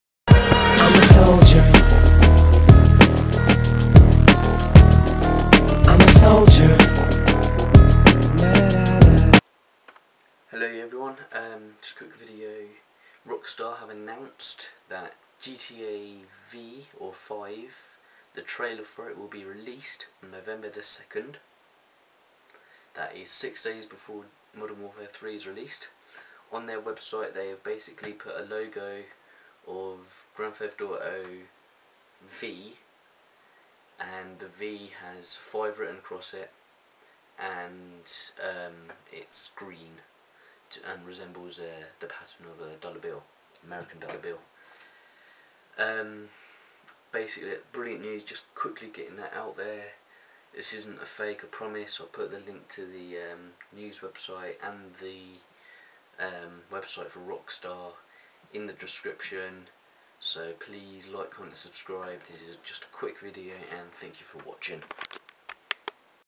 GTA V trailer announced
OMG OMG OMG rockstar have announced that the trailer for their upcoming game GTA V will be released on november the 2nd, thats 6 days before modern warfare 3 is released IM SO AMPED RIGHT NOW